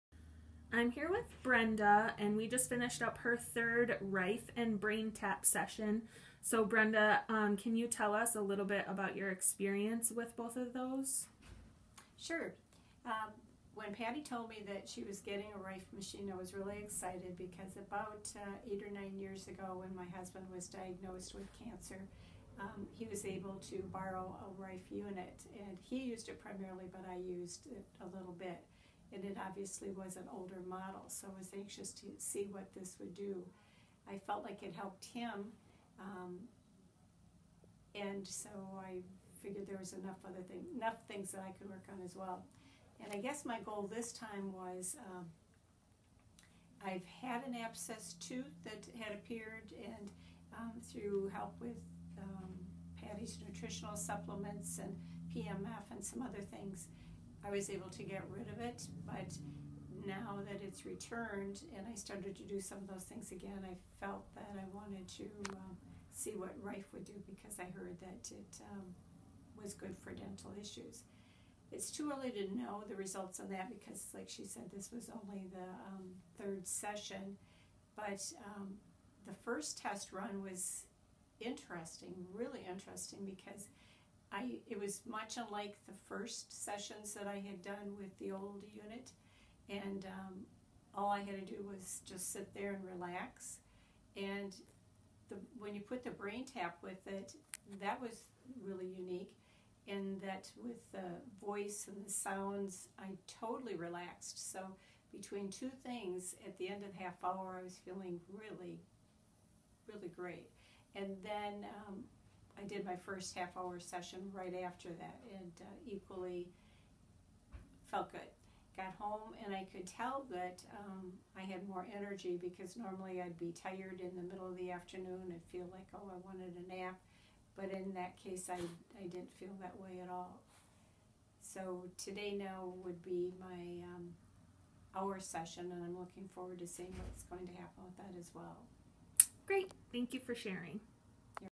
Brenda gets deeply relaxed and feels more energy with the Rife and BrainTap combo
"At the end of the half-hour it was really, really, really great!" "I could tell that I had more energy!"